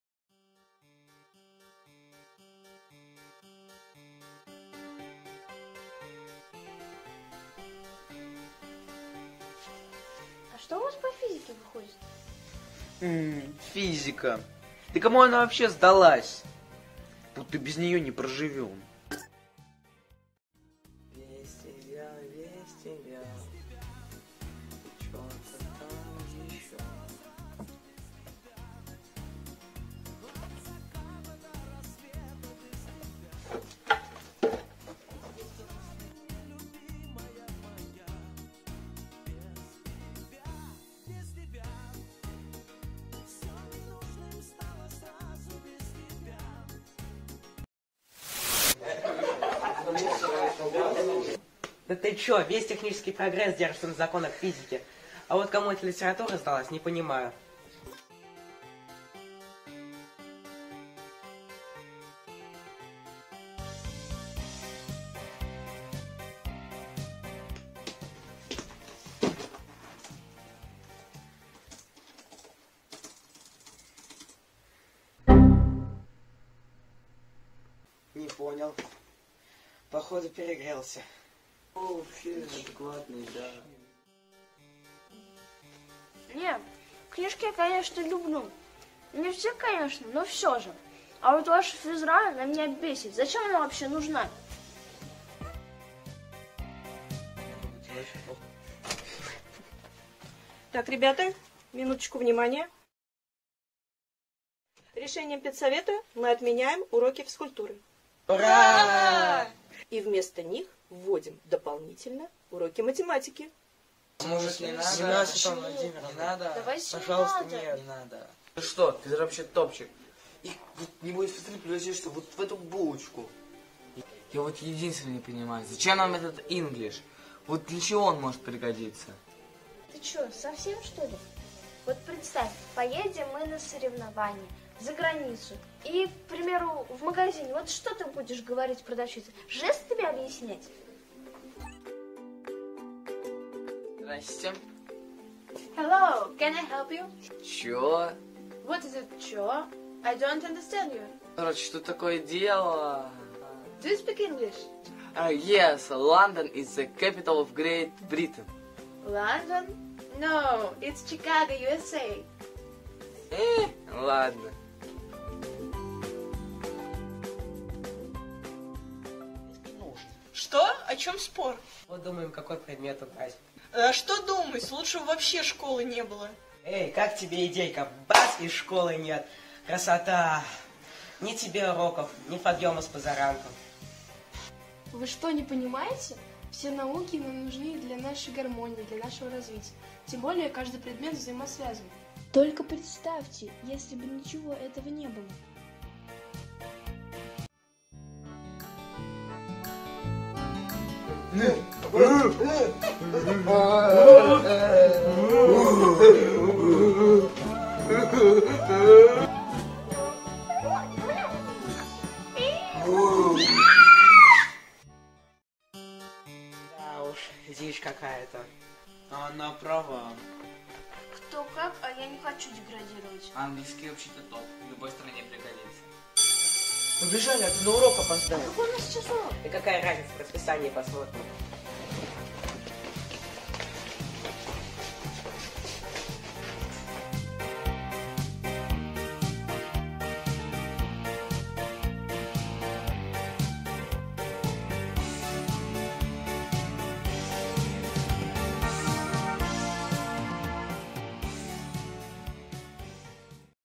"Мученики" науки
Образцовая детская студия фотовидеотворчества "Субъектив" МБУ ДО "ДШИ №3 "Младость" г.о. Самара на базе ГБОУ СО "ЛАП №135 (Базовая школа РАН)"
Г. Самара

Одноклассники на переменке мечтают об идеальной школе, предполагают, что бы в ней улучшить: убрать, упростить... В спорах и рассуждениях рождается истина.